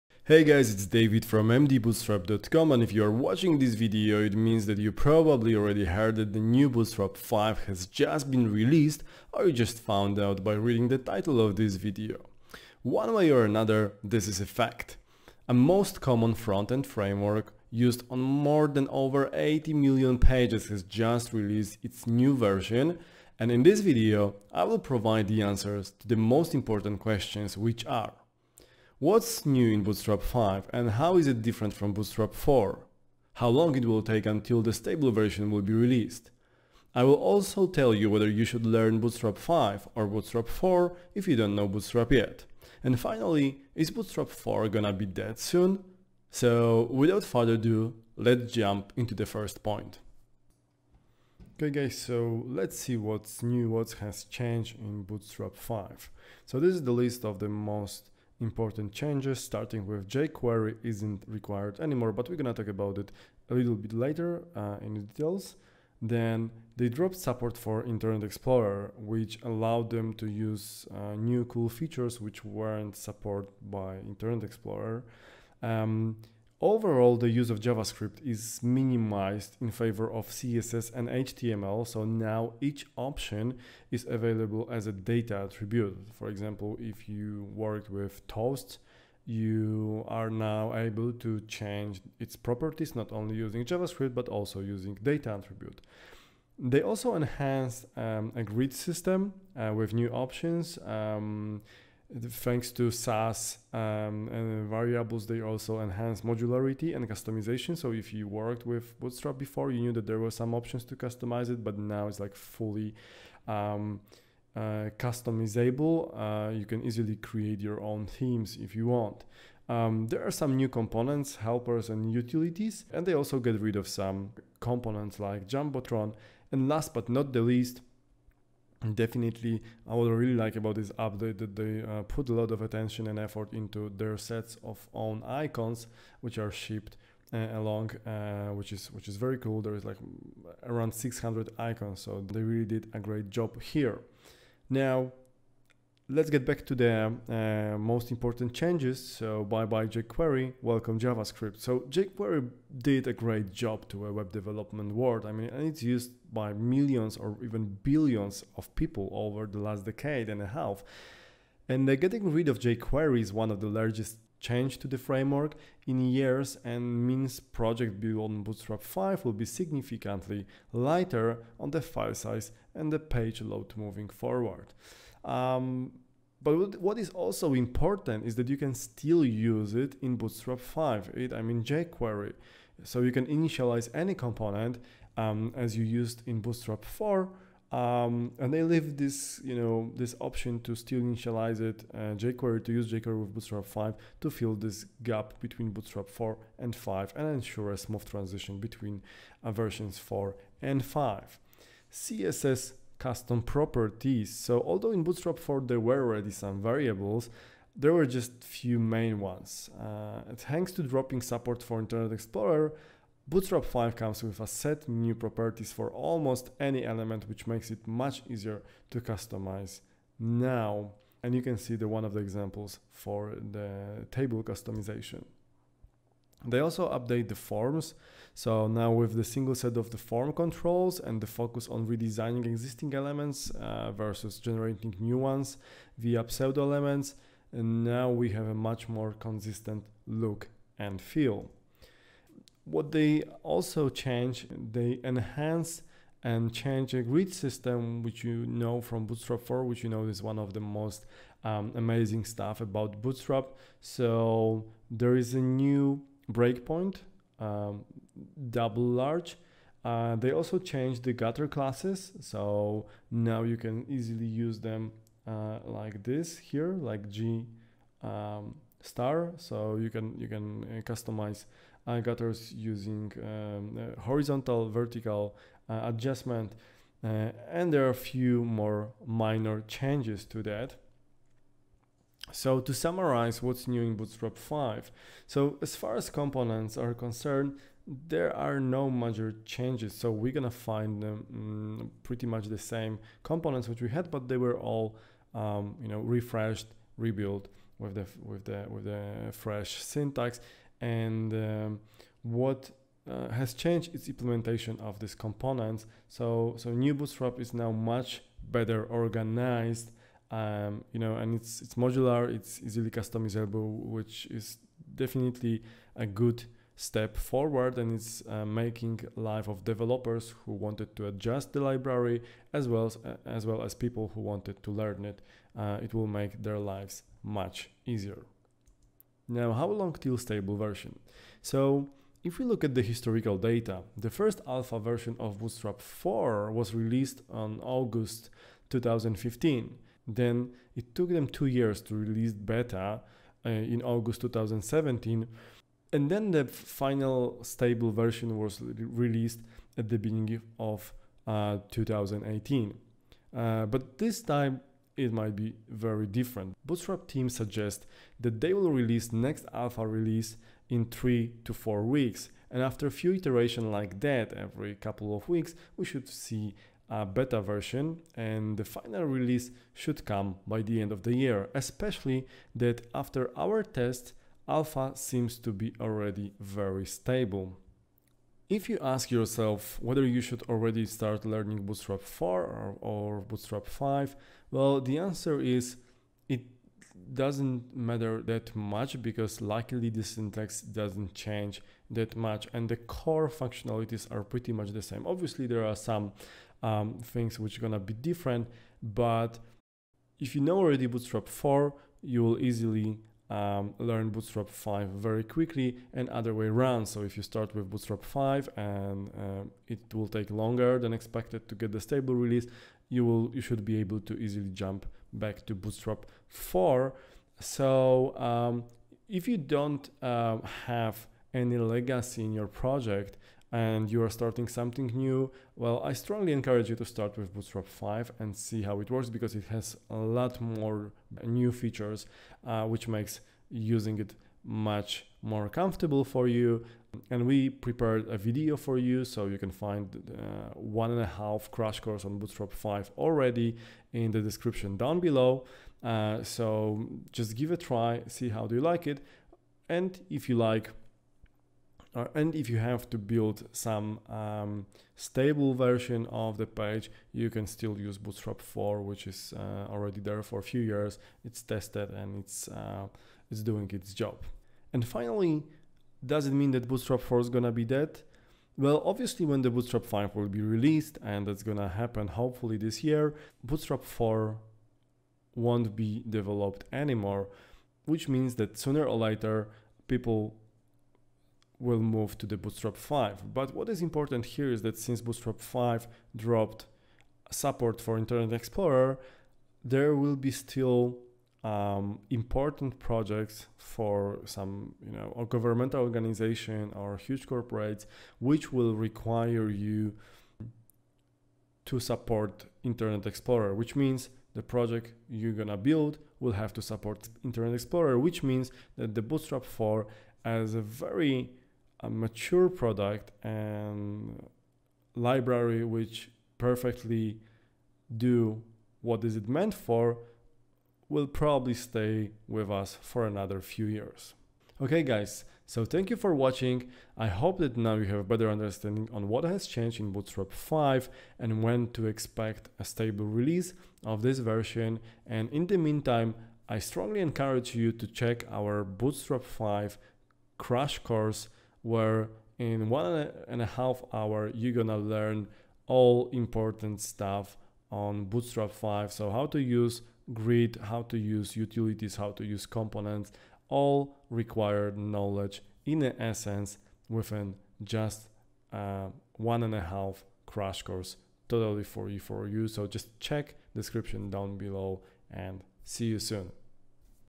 Bootstrap 5 - What's New? Summary, Download, Next Releases & Tutorial
In this video, I will provide the answers to the most important questions about Bootstrap 5, which are:

 ▶ what's new in Bootstrap 5 and how is it different from Bootstrap 4
 ▶ how long it will take until the stable version will be released
 ▶ whether you should learn Bootstrap 5 or Bootstrap 4
 ▶ it's bootstrap 4 gonna be dead soon

If you have any question, do no hesitate to join our Facebook group and ask: